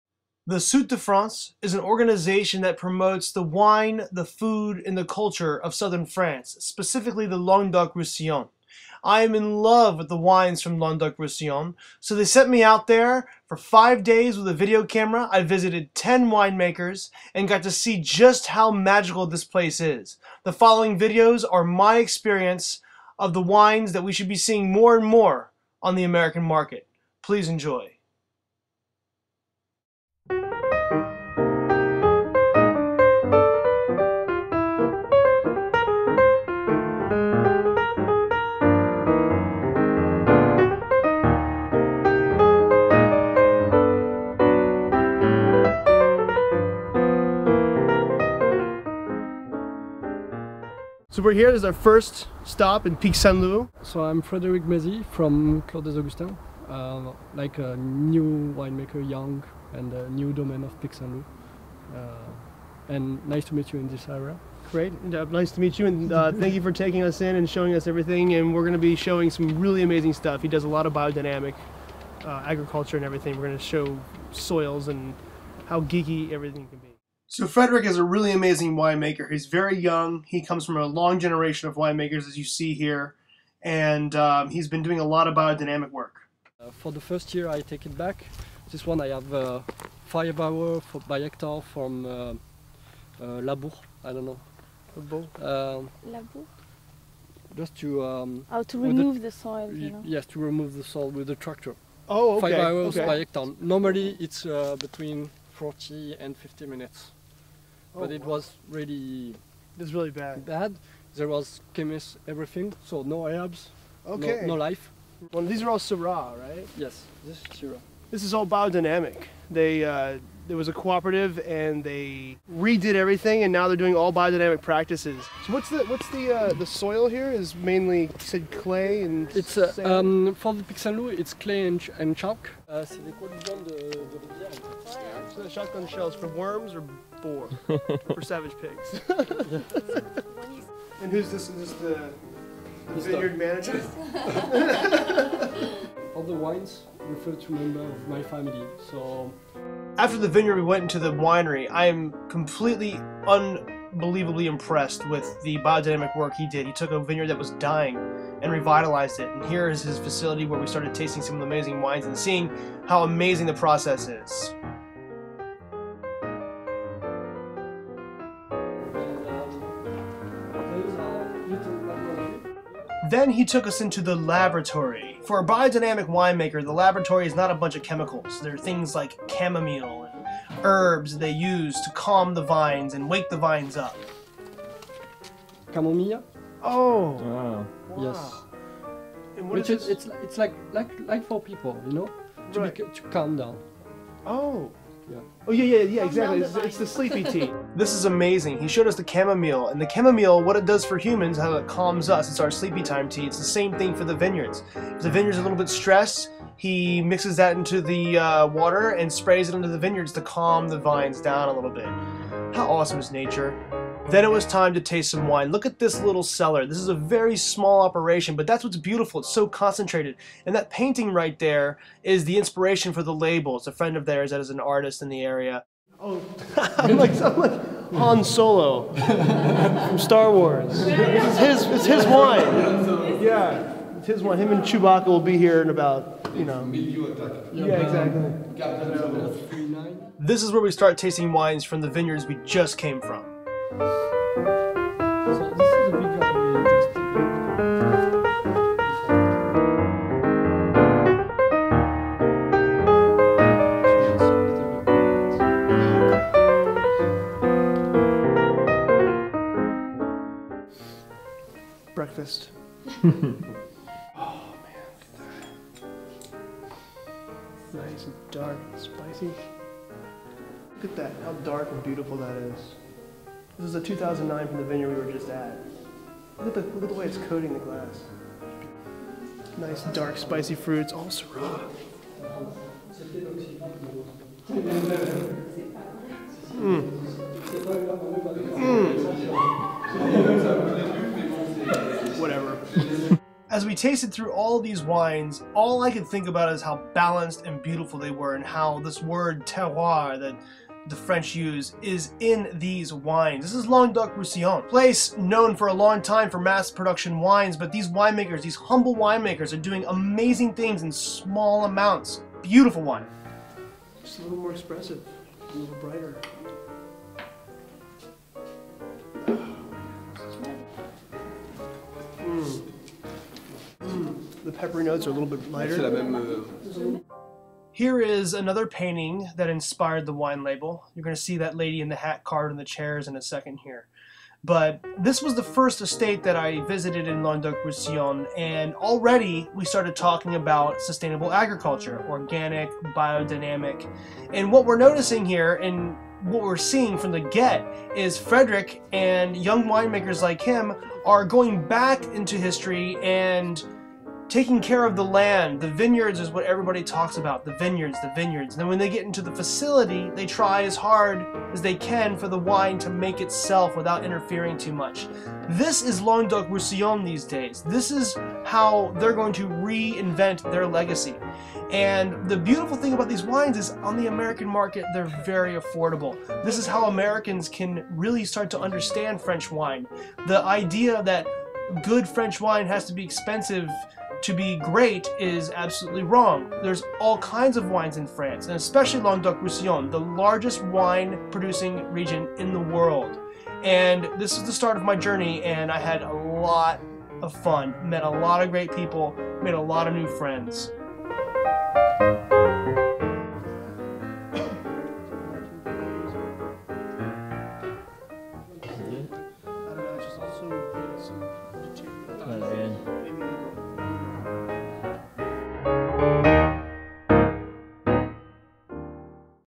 Clos Des Augustins
The Sud de France, an organization that promotes the wine, food and culture of southern France, specifically Languedoc-Roussillon, caught wind of my obbession with the area and began watching my video series on the many regions from west to east.

They asked if I would take my video camera to France and capture the magic first hand. I said. YES! YES!YES!

Over the next five days I experienced something truly special. We travelled from the most eastern border of the this immense appellation to Pyrenees bordering on Spain.